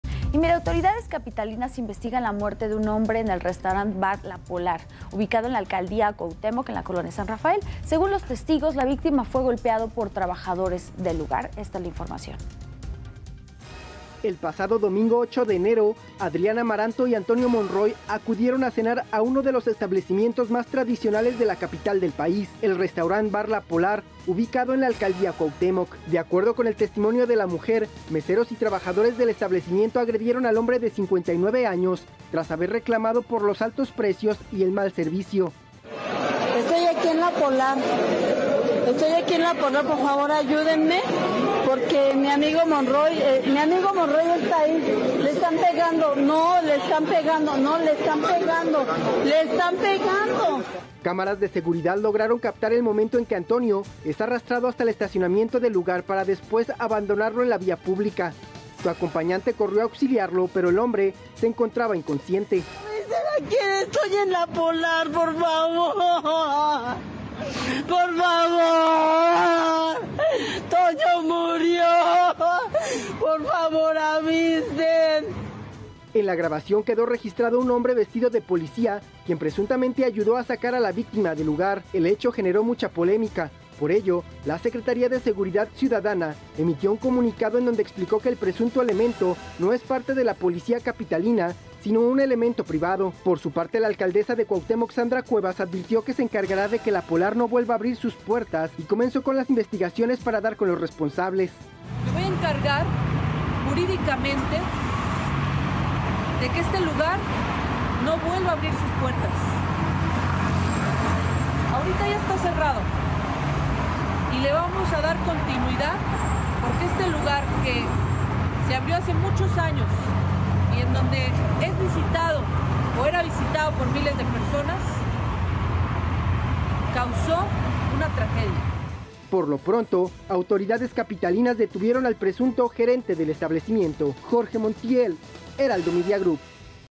Bar la Polar: Fiscalía de la CDMX investigará agresiones a cliente como homicidio doloso
La noche de este lunes 9 de enero la Fiscalía General de Justicia de la Ciudad de México (FGJCDMX) informó que investigará como homicidio doloso la muerte de un cliente en el Restaurante "La Polar", ubicado en la colonia San Rafael, alcaldía Cuauhtémoc. Agentes del Ministerio Público de dicha demarcación tomaron conocimiento de los hechos ocurridos el pasado domingo luego de que se difundieran videos del caso.

De acuerdo con las primeras versiones, un hombre identificado como Antonio Monroy Jiménez, de 59 años se encontraba bebiendo con una acompañante y tuvo un desencuentro con los meseros, por lo que fue agredido físicamente hasta la muerte. La víctima fue arrojada en calle Guillermo Prieto donde fue localizado por elementos de la Secretaría de Seguridad Ciudadana (SSC), quienes lo canalizaron a un hospital, sin embargo, fueron los cuerpos de emergencia los que lo declararon muerto.